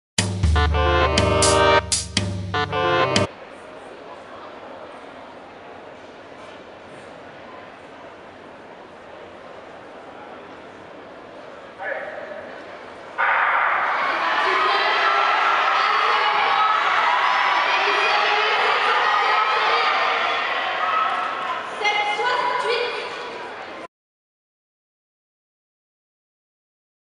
Belgian athletics U16 indoor championships 60m women Final
1 4 1531 MORTANT ALEXANDRA  CABW Cad 7.68 CB 1
2 5 2114 NZUKOU WAKAM CALLIE LIZZI  MOHA Cad 7.72 CB 2
3 1 1847 JEHAES MARINE  SER Cad 7.98 CB 3
4 6 528 SEGHERS EVY  OEH Cad 7.98
5 3 162 CLAES NOA  VMOL Cad 7.98
6 7 2071 MPINGA MULOWAYI ALEXIA  RESC Cad 8.03
7 2 1573 COLLARD ERINE  ARCH Cad 8.04
8 8 204 DE BLOCK VEERLE  ACW Cad 8.13